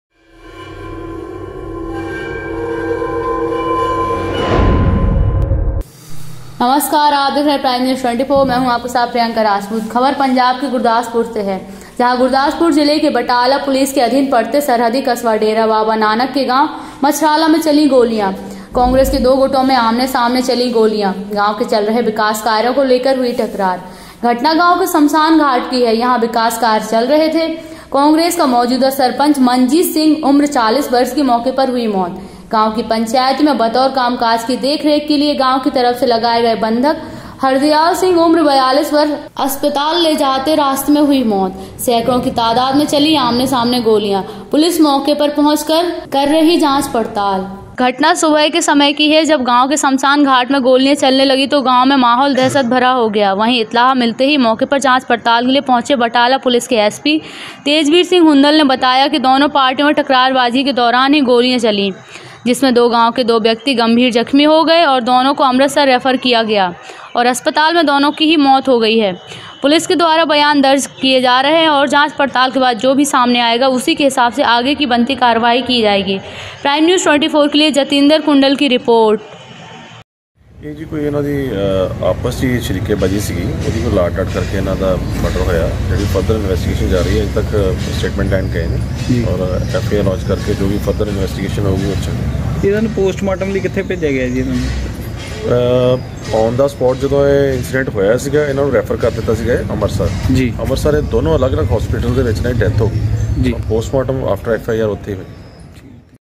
तकरारबाज़ी के दौरान कांग्रेस के 2 धड़ो में चली गोलिया ,गांव के सरपंच और गांव के एक व्यक्ति की मौत
तकरारबाज़ी के दौरान कांग्रेस के दो धड़ो में चली गोलिया ,गांव के मौजूदा सरपंच और गांव के एक व्यक्ति की मौत